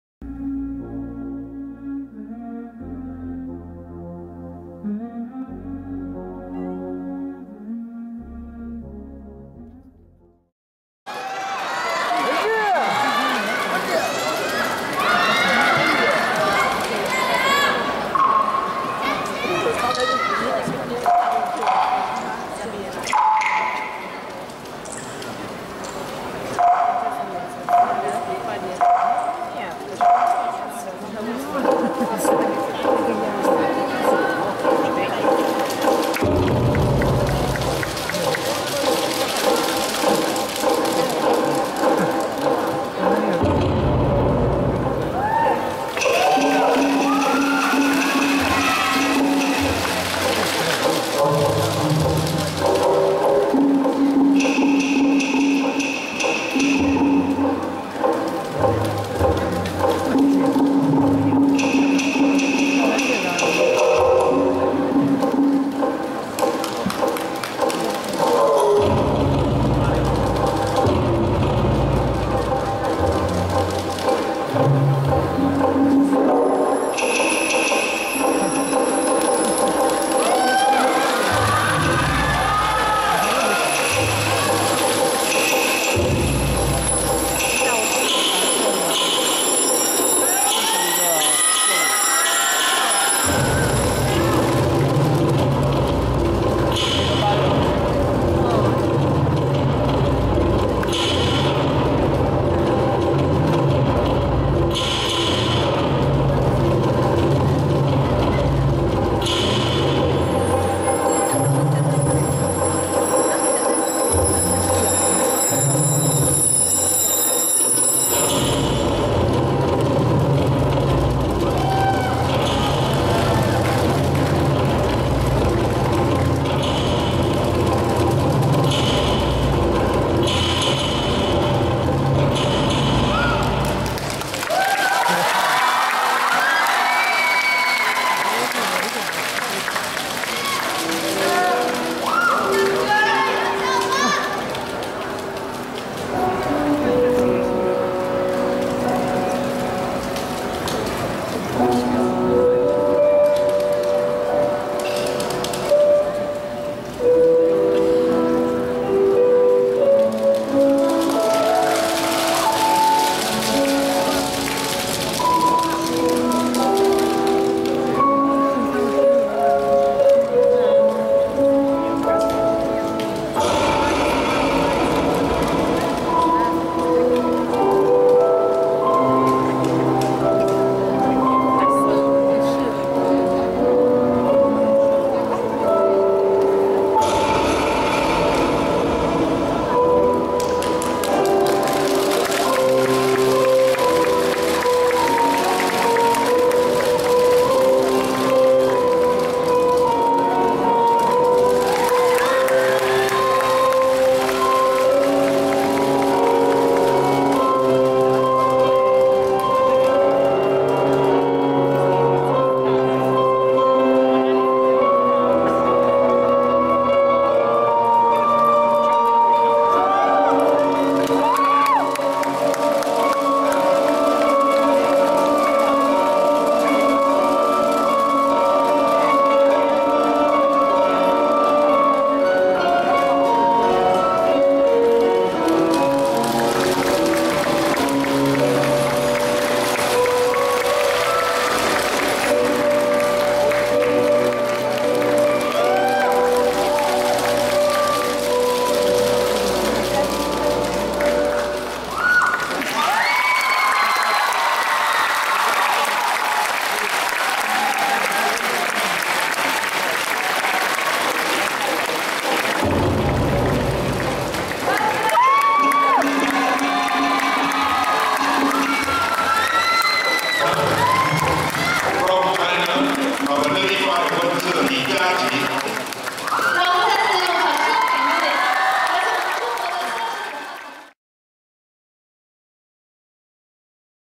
Wen Zhi - Jiaqi Li | 2012 WDSF World ShowDance LAT - Final
Wen Zhi & Jiaqi Li, CHN, dance their routine in the final of the 2012 World DanceSport Championship Freestyle Latin in Beijing, CHN.